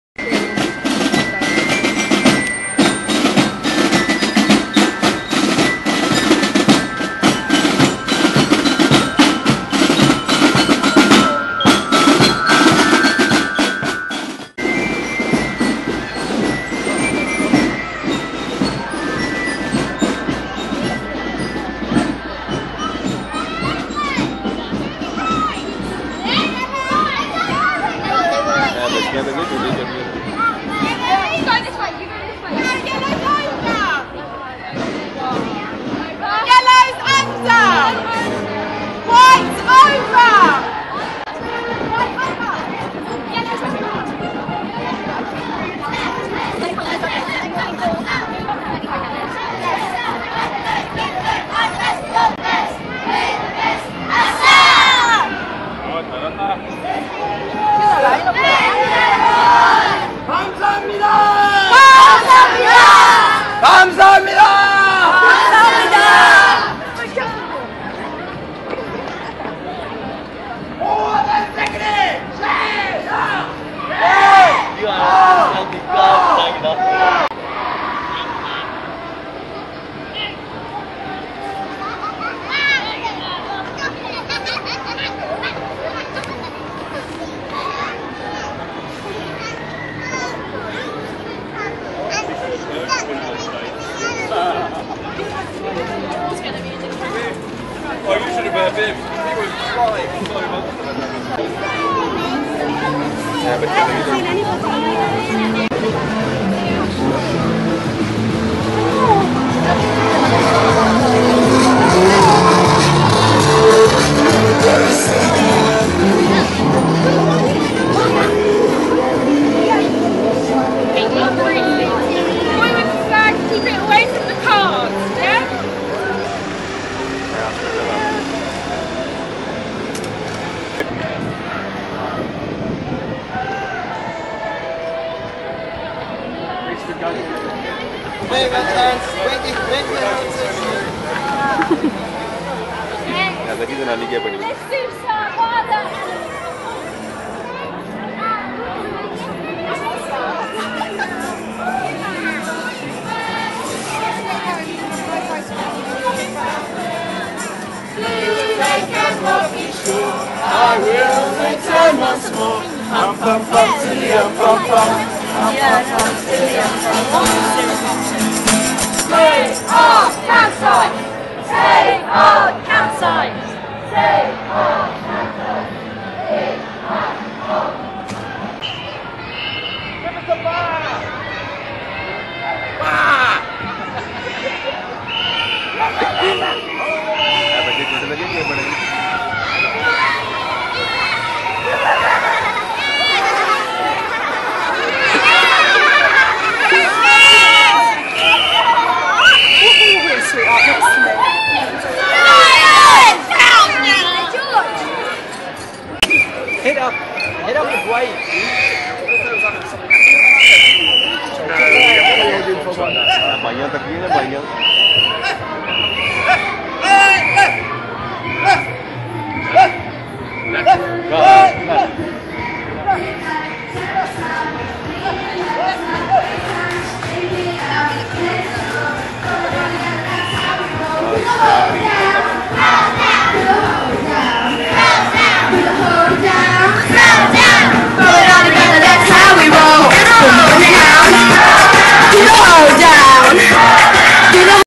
06-07-2013
The parade to signal the start of the New Malden Fortnight